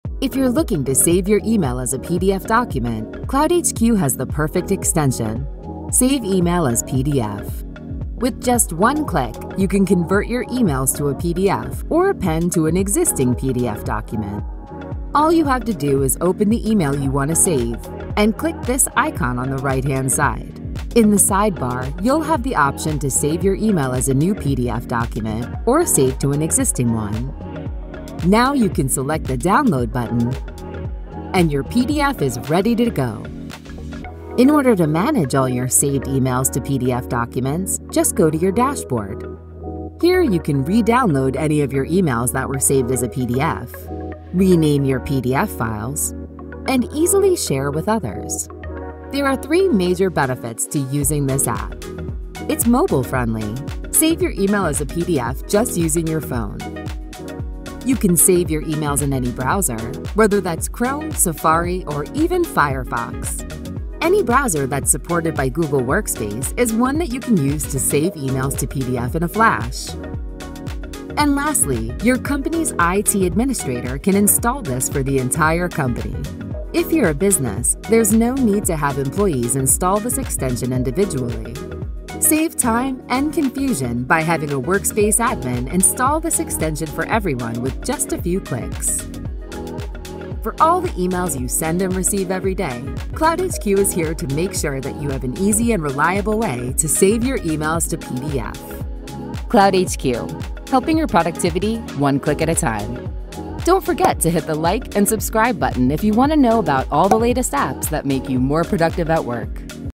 Benefits of Saving Email Messages to PDF
~~~ Why save emails as PDF? ~~~
- Searchability: PDF readers can offer you excellent search results within  seconds of a query in large files.
- Compatibility: Format, text, images, and hyperlinks remains intact regardless of software of hardware that anyone views it on.
- Accessibility: Find everyone whether you have a wifi connection or not
- Preserved Format: For legal issues, a PDF of your emails is a widely accepted format that it has been untampered.
- Security and Privacy: You can encrypt and password protect your file.